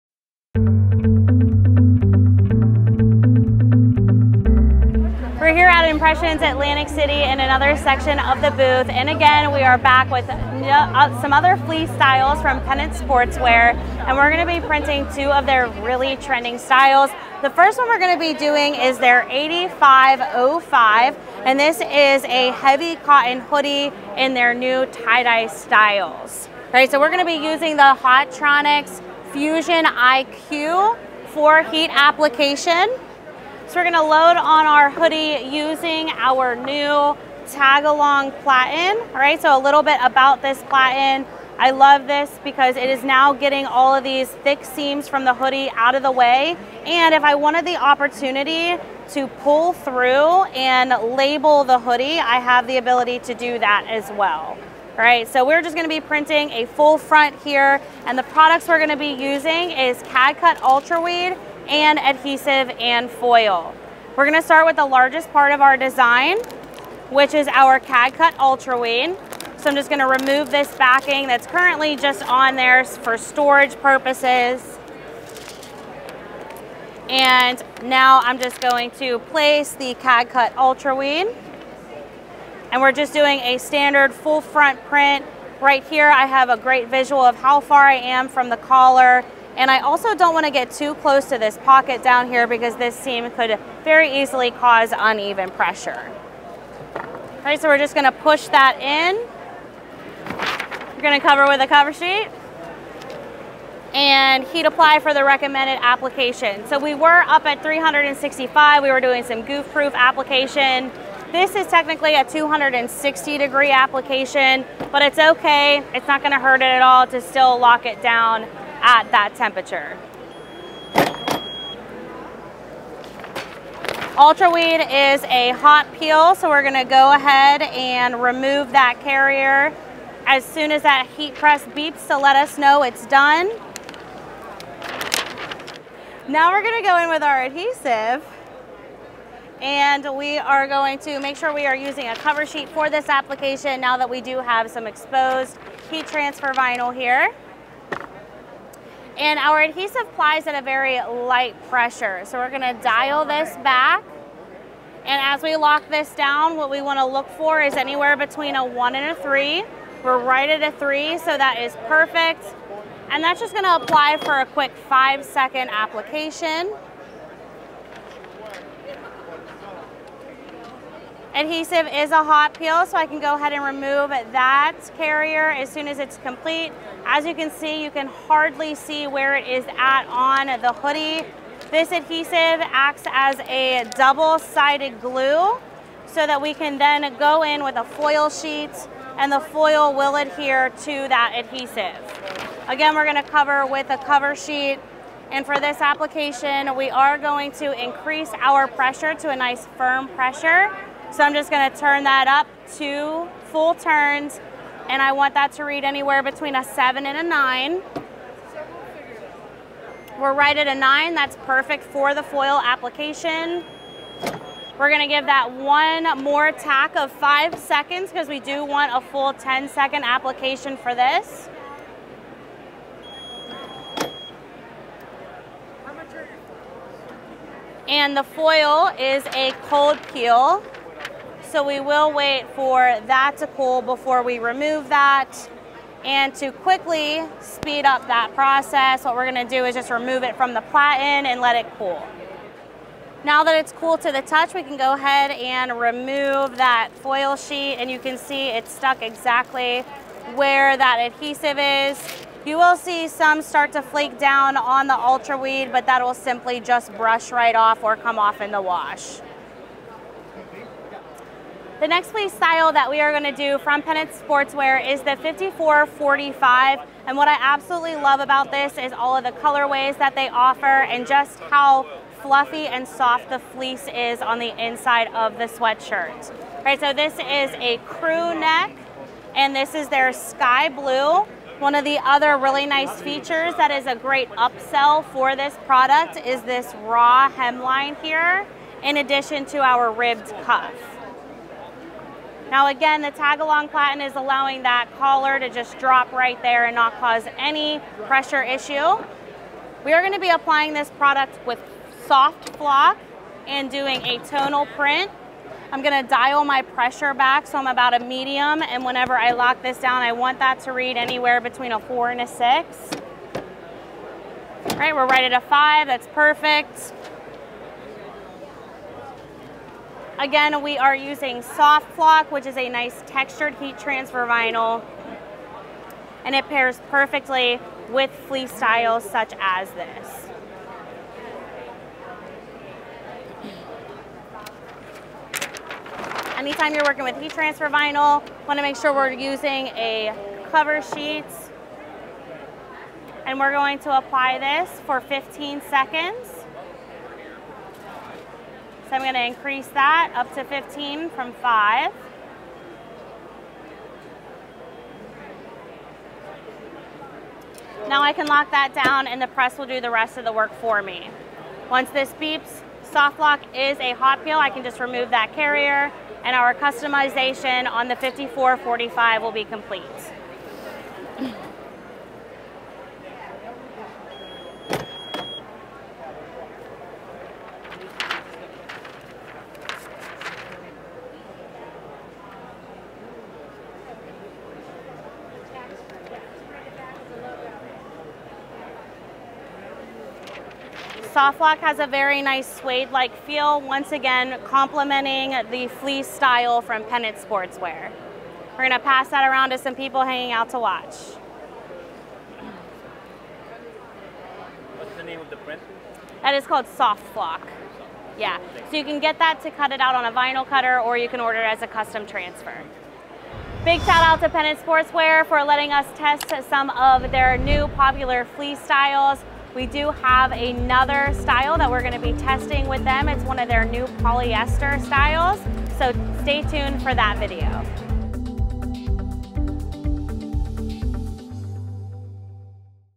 Best Heat Transfer for Fleece Apparel Pt. 2
Do you still need help finding the perfect heat transfer vinyl to pair with the fleece apparel? Well, fortunatelyJenna is back with part 2 of our "Best Heat Transfers for Fleece Apparel" series to give you a few more HTV options to consider. Get a first-hand look at how fleece sweatshirts from Pennant Sportswear can be decorated with heat transfer vinyl like Stahls' CAD-CUT® UltraWeed, heat transfer foil, and even CAD-CUT® Soft Flock with ease.

Need help finding a local STAHLS'® CAD-CUT® HTV reseller near you? Simply click the link here to find the closest CAD-CUT® reseller in your city today